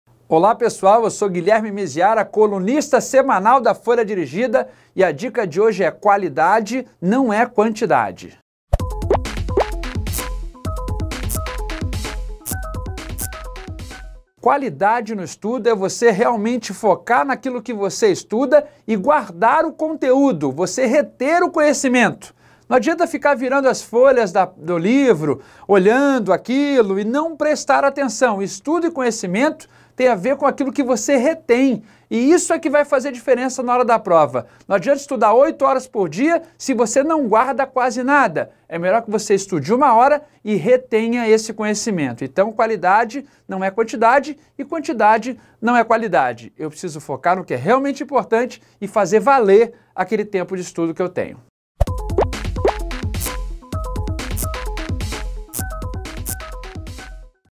Como ter estudo de qualidade para concurso?
Como ter estudo de qualidade para concurso? Esta é a sua dúvida? Neste vídeo Guilherme Miziara traz orientações para que o seu 2019 seja de estudo correto e boas provas!

Seja um assinante FOLHA DIRIGIDA!